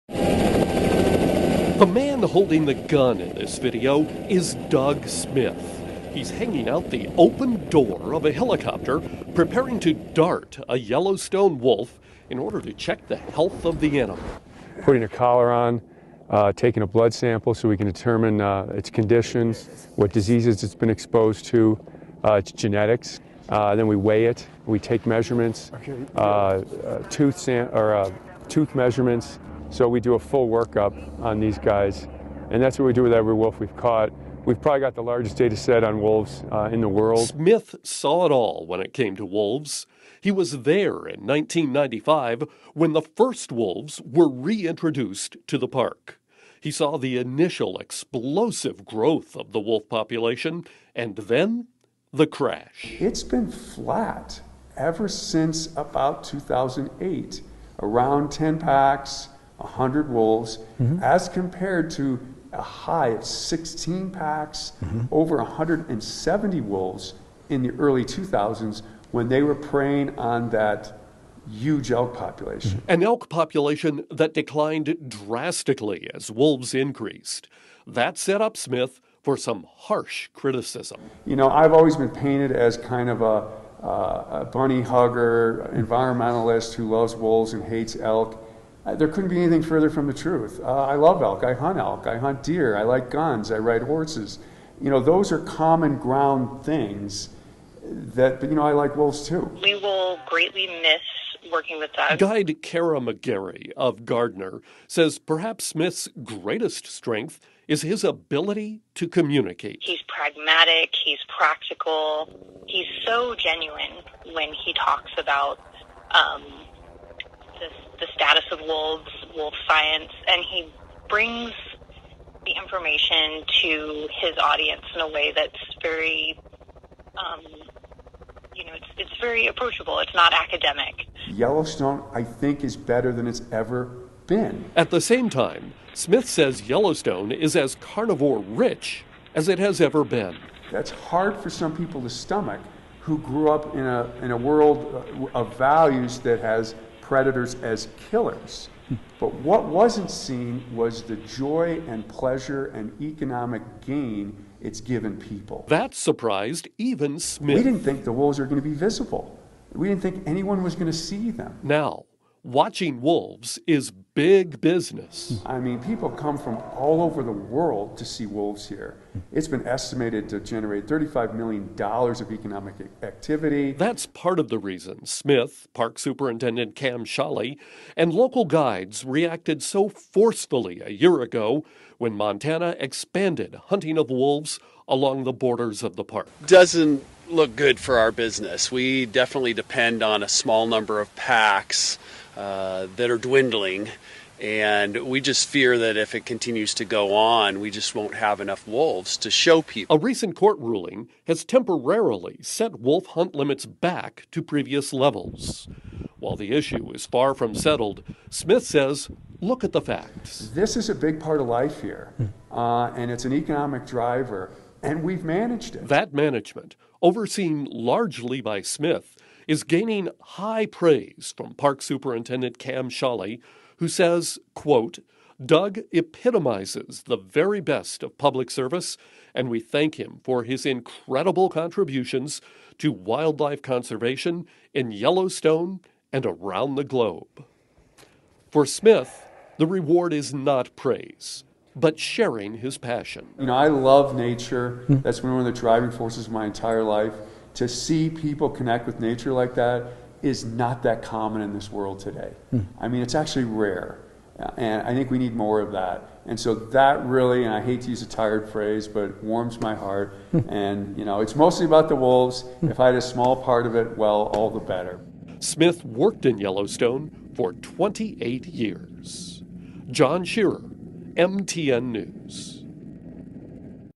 Retiring Yellowstone biologist reflects on wolf management in the park
After a 28-year career in Yellowstone National Park, biologist Doug Smith looks back on the successes and challenges of wolf management in the park.